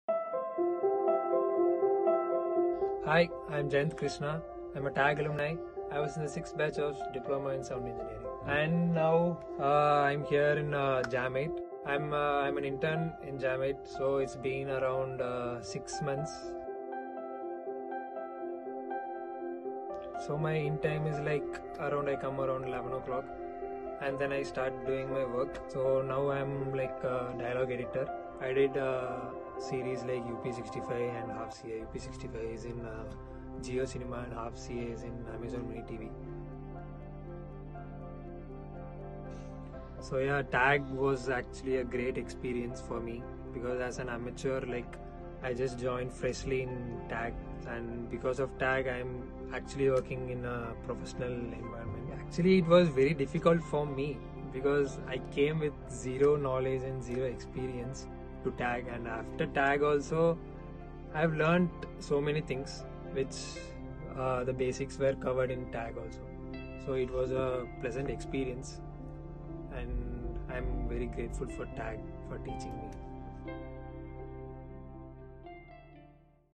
TAG Alumni- Jayenth Krishna | Jam8
Batch 6 Jayenth walks us through his experience of studying at the audio guys institute, explains us the key skills he learned at TAG to put to use at his place of work at JAM8 studios as a dialogue editor.

Series like UP65 and Half CA have been a part of Jayenth's Portfolio. First of many we are sure!